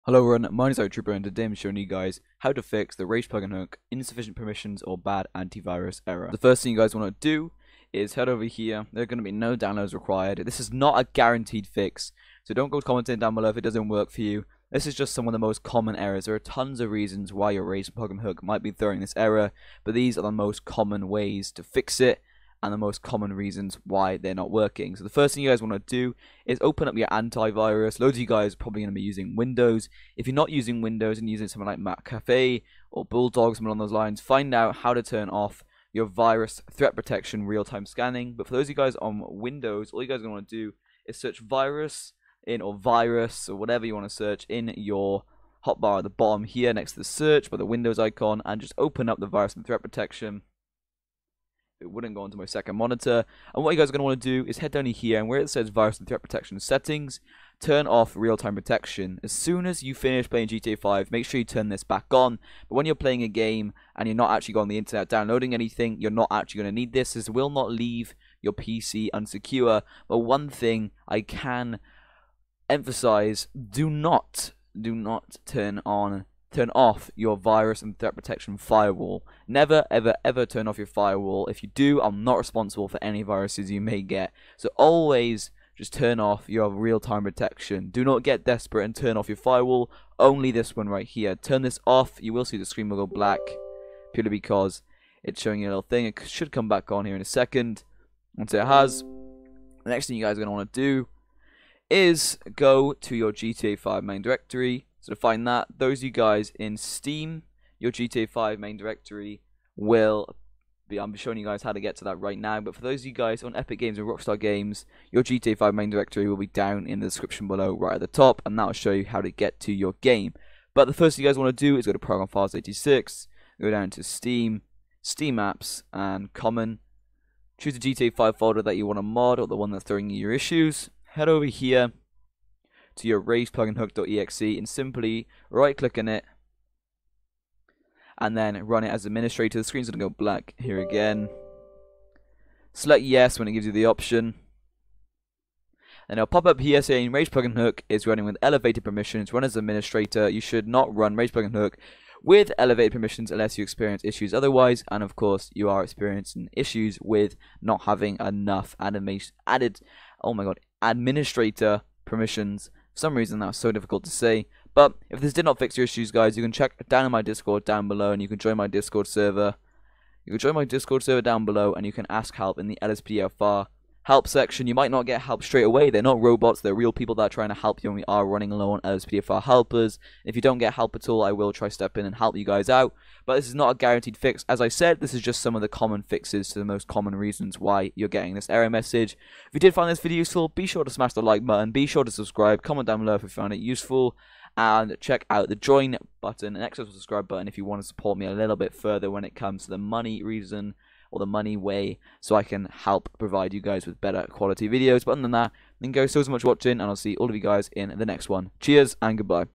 How to FIX/Stop Rage Plugin Hook insufficient Permissions and Bad Antivirus Error Message
If you want to support me further for extra special rewards-
Epic-
ProgramFiles/Epic Games/GTA V
Rockstar-

Downloads-